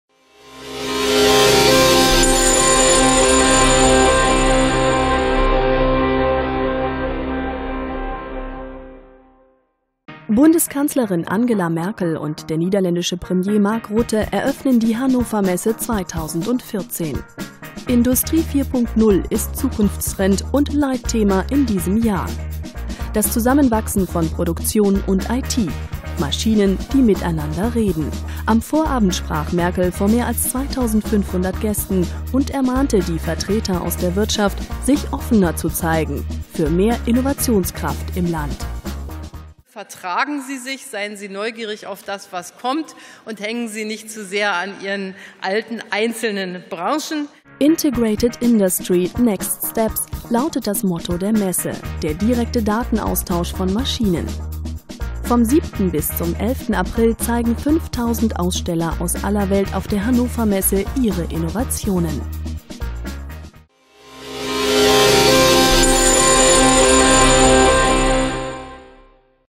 HANNOVER MESSE 2014 - Eröffnungsfeier (deutsch)
Bundeskanzlerin Angela Merkel und der niederländische Premierminister Mark Rutte eröffnen die HANNOVER MESSE 2014.